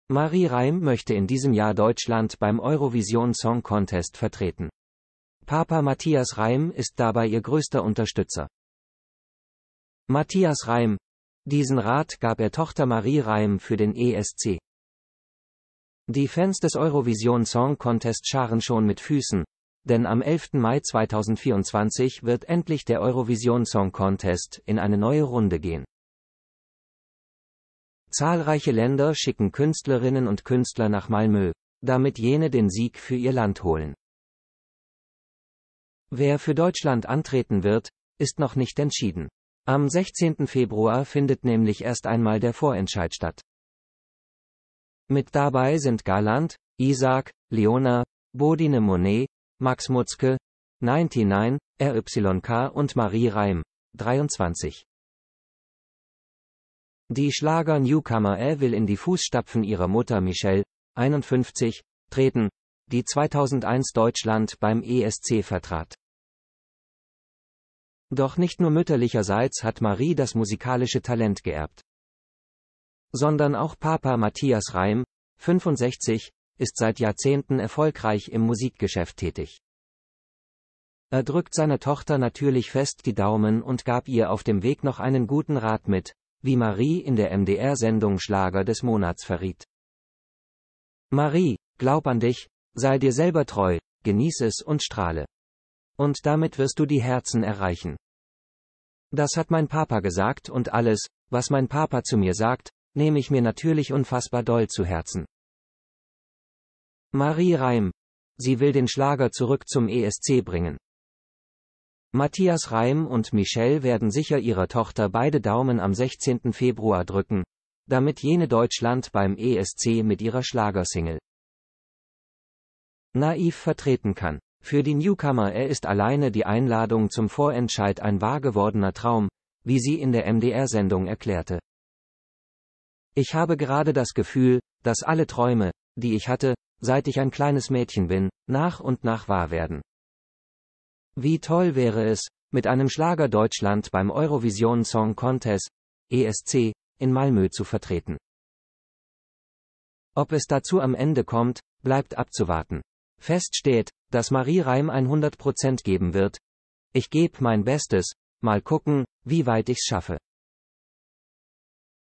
Matthias Reim Rührende Botschaft!
Marie Reim möchte in diesem Jahr Deutschland beim Eurovision Song Contest vertreten. Papa Matthias Reim ist dabei ihr größter Unterstützer.